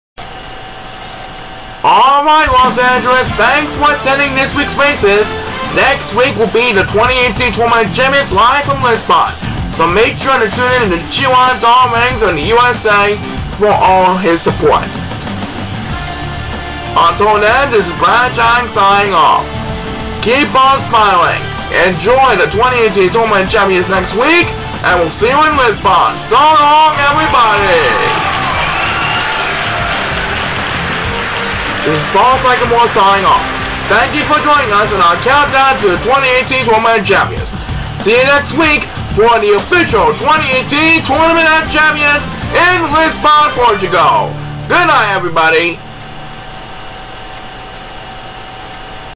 Chao Racing 2018 Week 22 Closing
NEXT WEEK: Find out which nation will emerge victorious in the 2018 Chao Racing Tournament Of Champions live from Lisbon, Portugal! Don't miss it then!